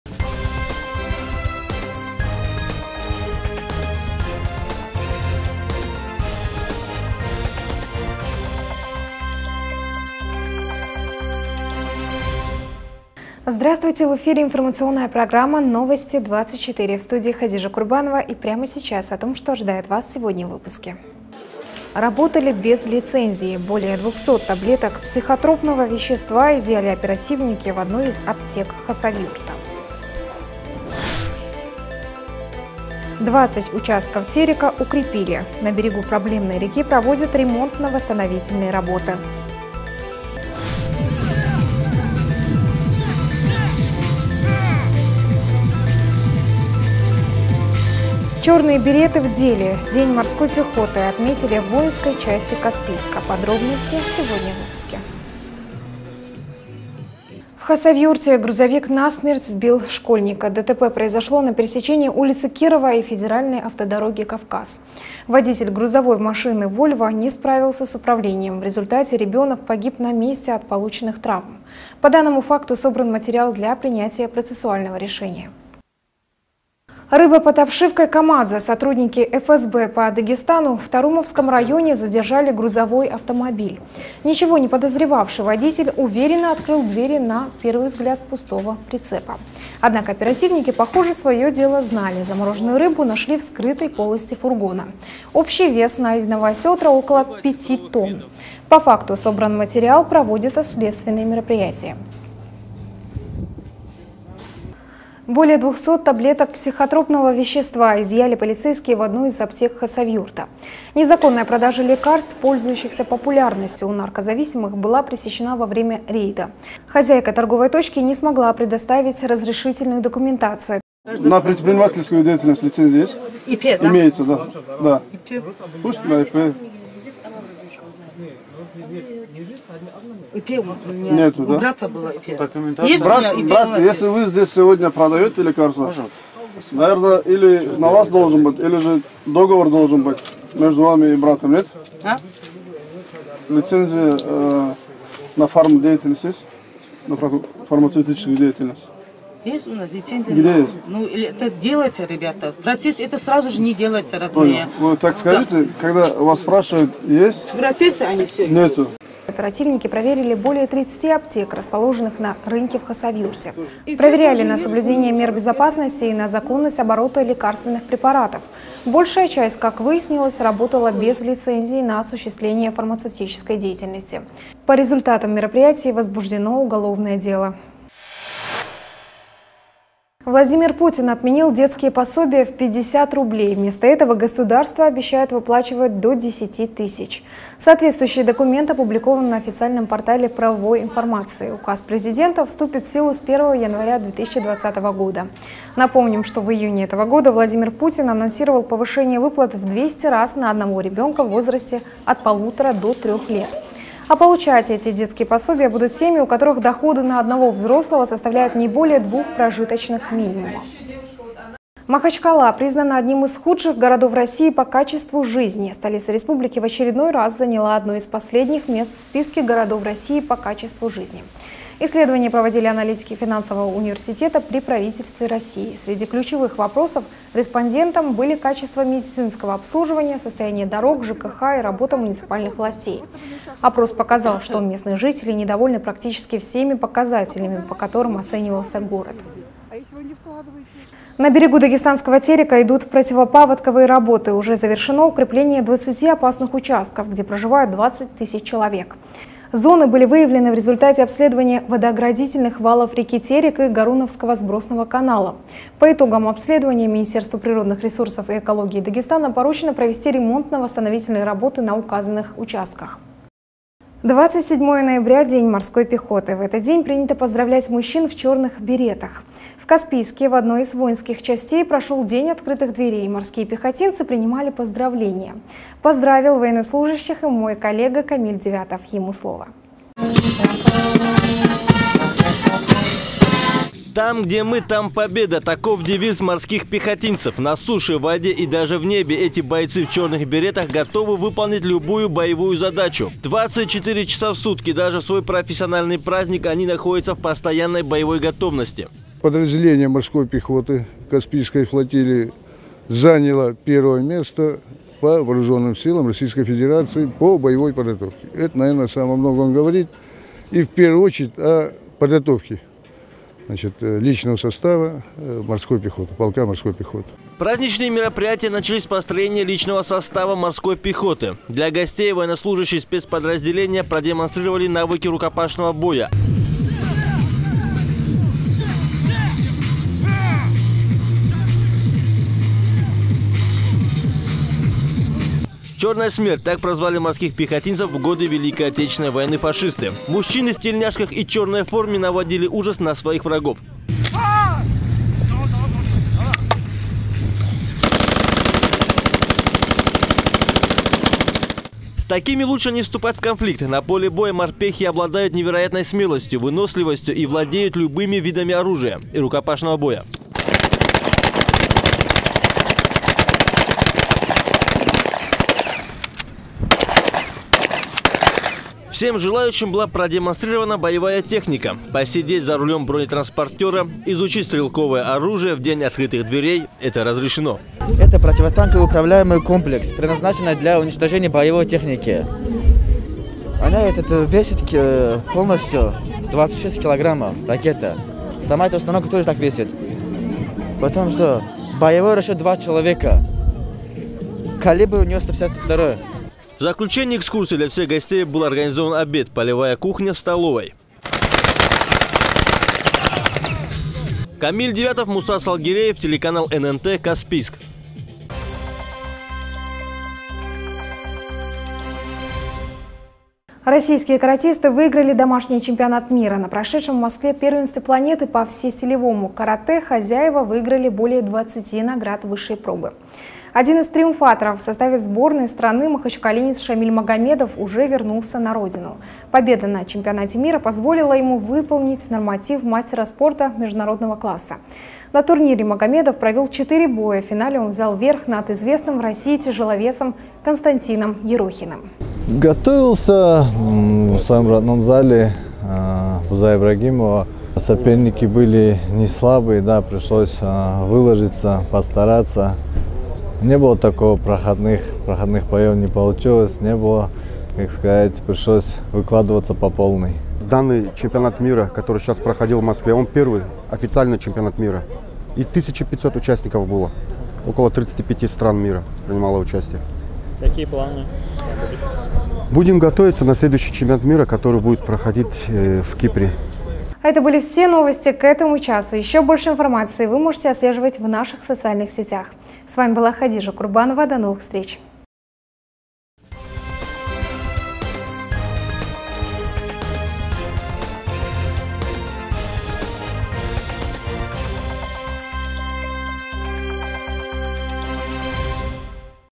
Новости Дагестана за 27.11.2019 год
1. Работали без лицензии. Более 200 таблеток психотропного вещества изъяли оперативники в одной из аптек Хасавюрта.
2. 20 участков Терека укрепили. На берегу проблемной реки проводят ремонтно-восстановительные работы
3.Черные береты в деле. День морской пехоты отметили в воинской части Каспийска. Подробности сегодня в выпуске